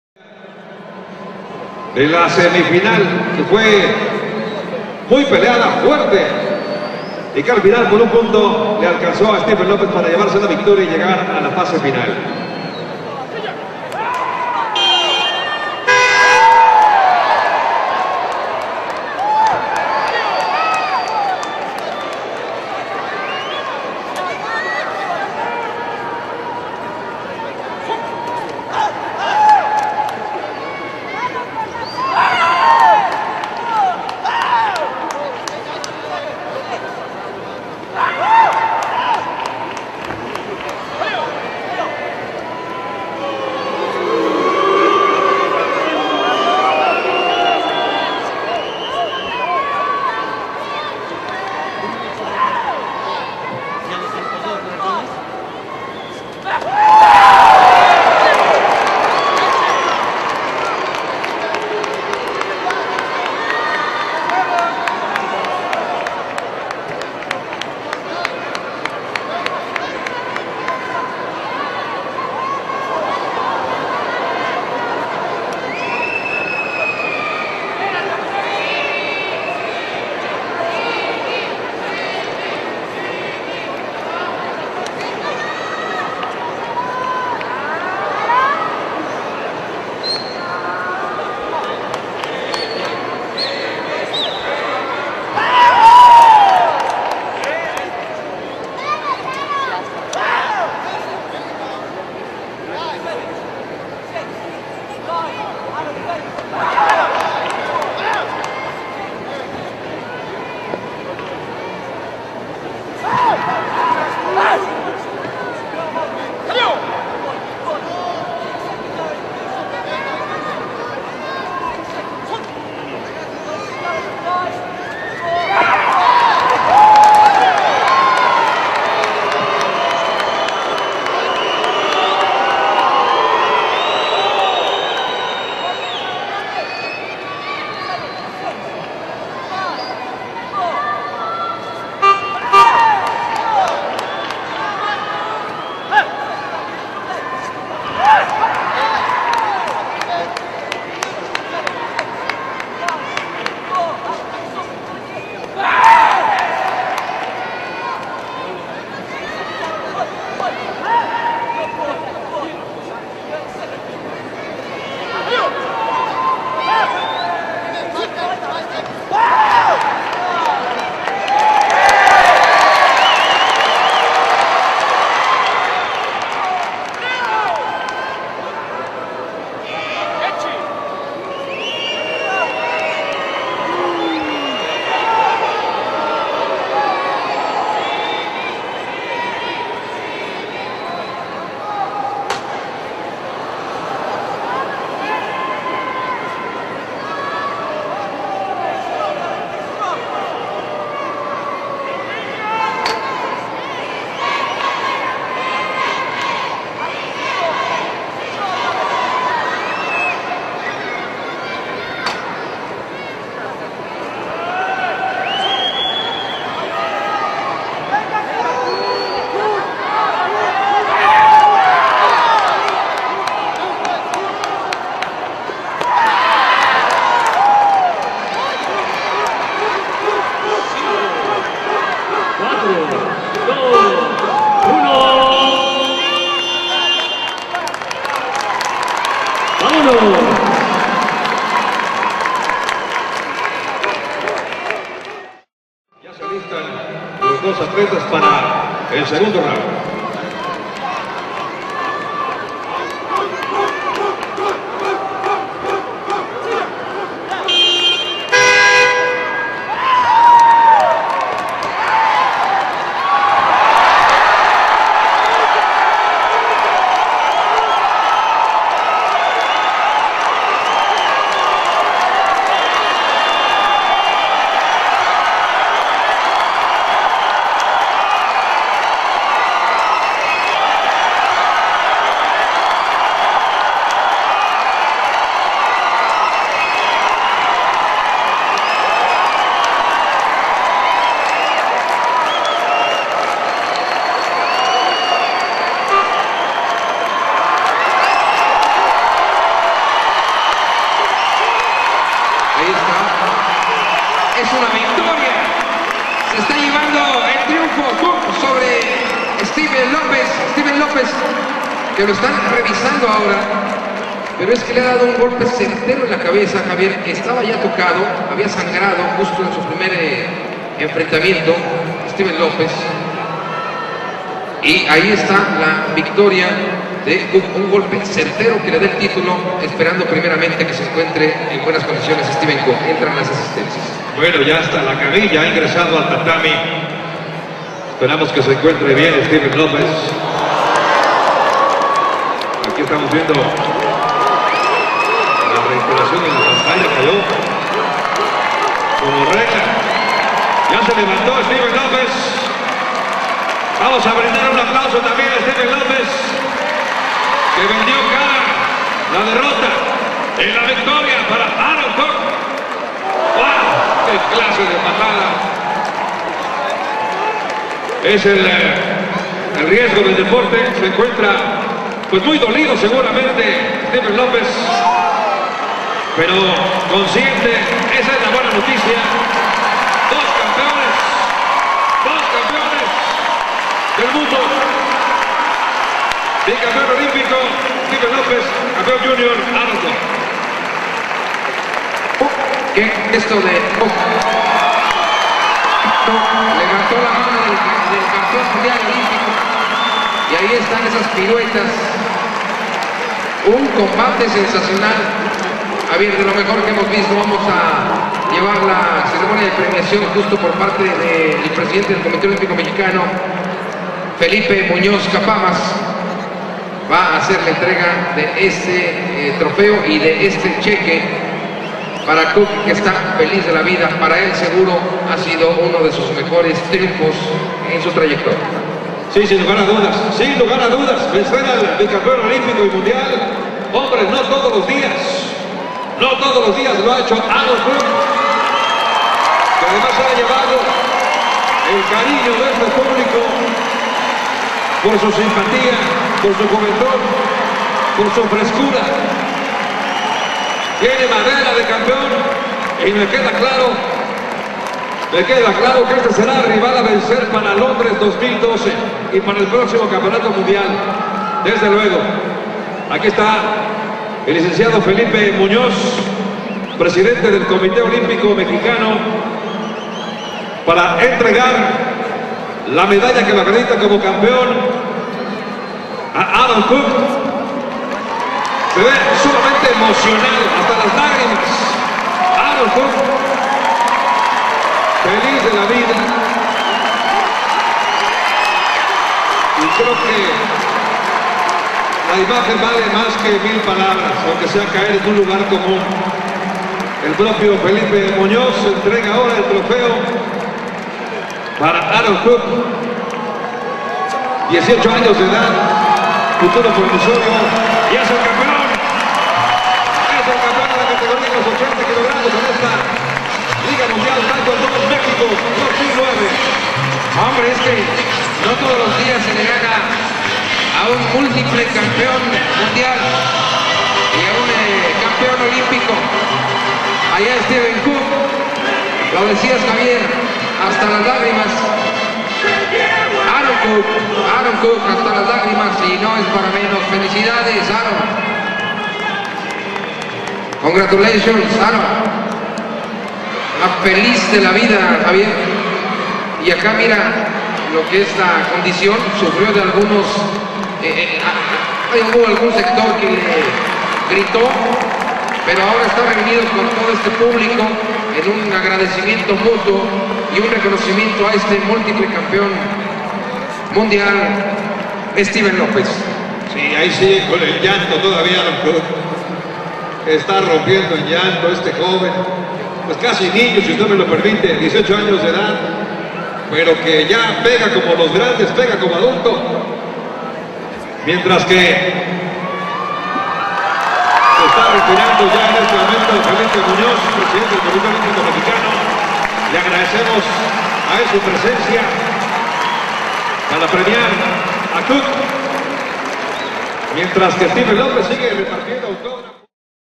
Aaron Cook Vs. Steven López (Full Video)
The full length video of Aaron Cook (GBR) vs. Steven López (USA) which was done during the World Taekwondo Tour in Mexico City, Mexico (Nov '09)